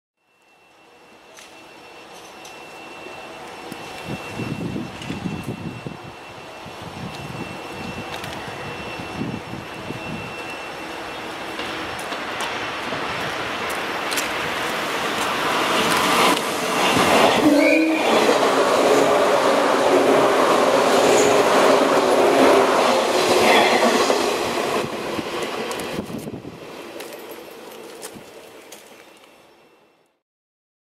【警笛あり】117系S1編成 回送
警笛を鳴らして山科駅を通過する117系S1編成です。